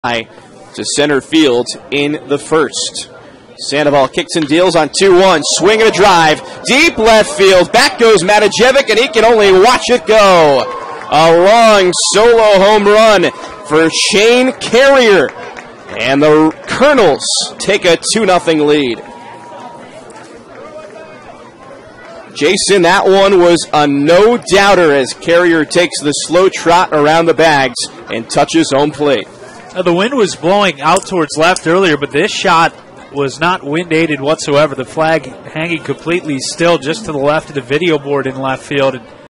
Carrier gives Cedar Rapids 2-0 lead in fifth.
4/5/18: Cedar Rapids' Shane Carrier hit a solo shot in the fifth to give the Kernels a 2-0 lead.